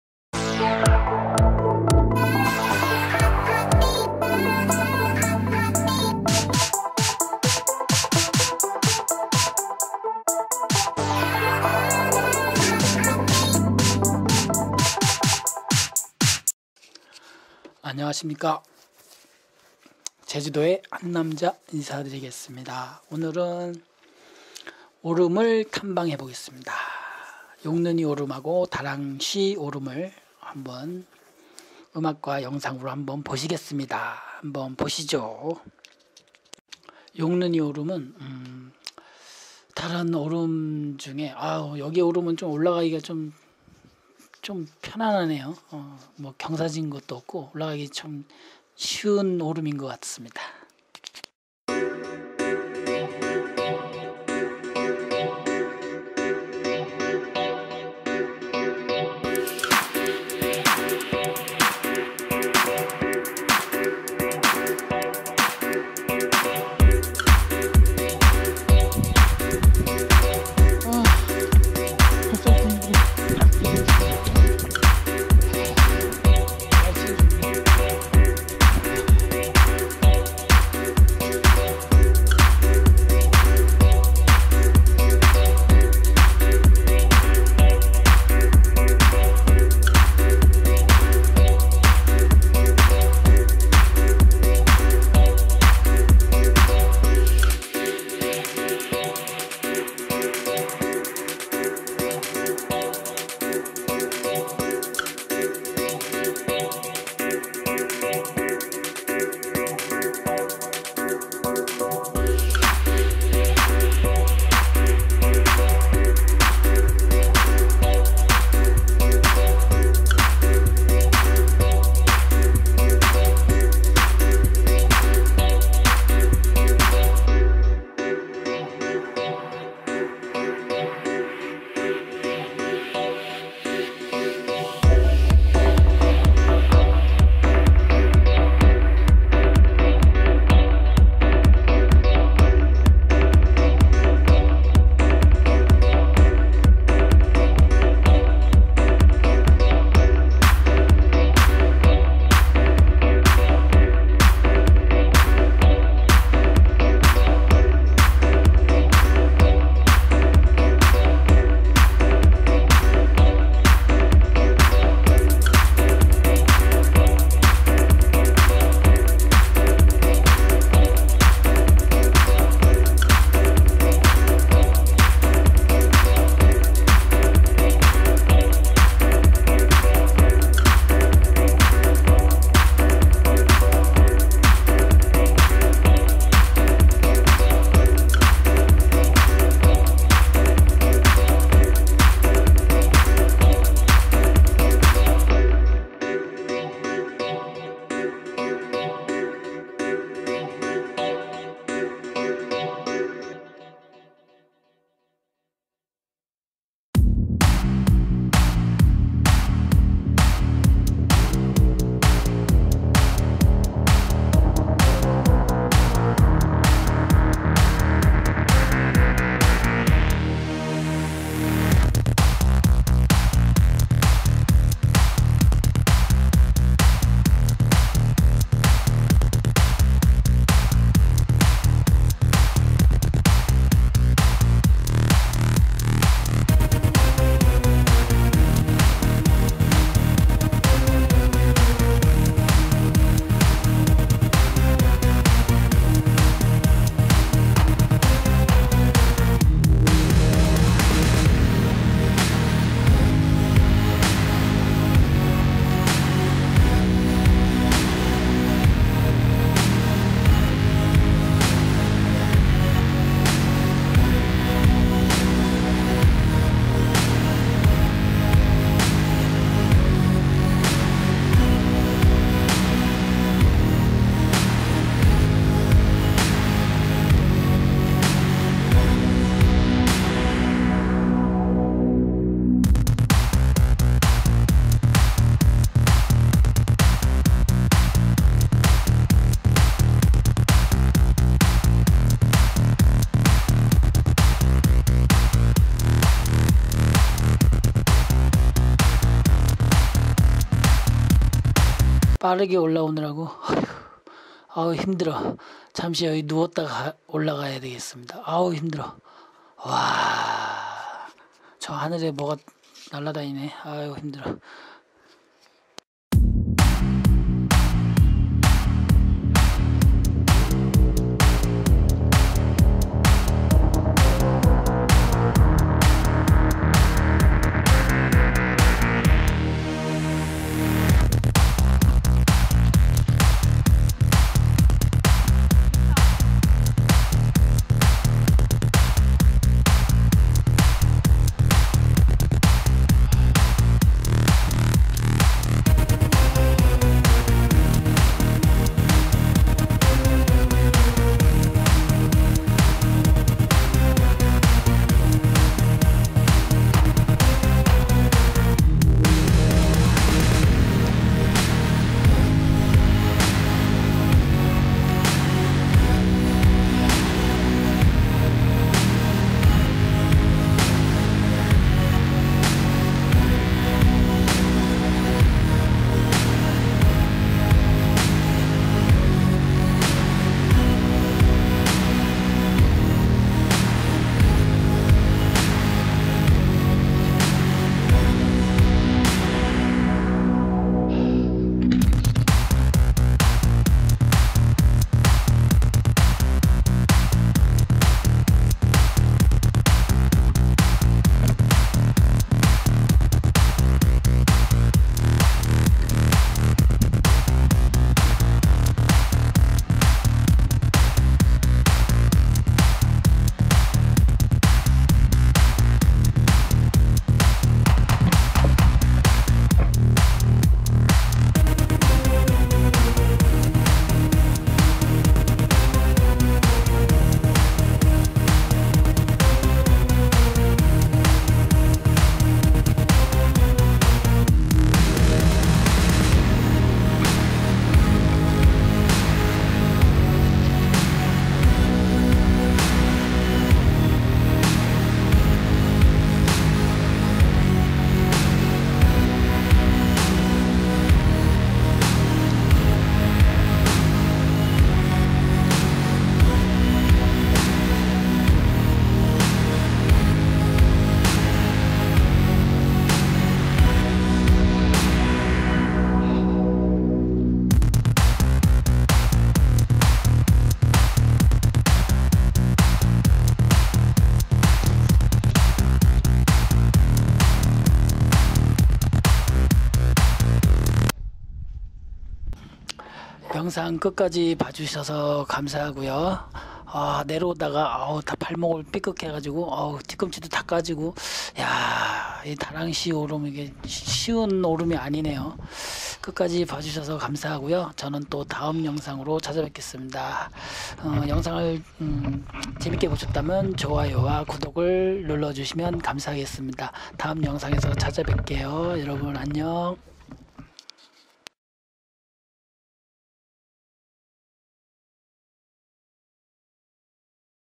용눈이오름&다랑쉬오름
전에 촬영한 파일이 나라 간줄 알았는데 다행이 복원 하여 다시 영상 올립니다.
이날 참으로 힘들게 올랐던 다랑쉬 오름 너무 힘드네요 !!!
진짜 힘듬 ㅎ;;;;;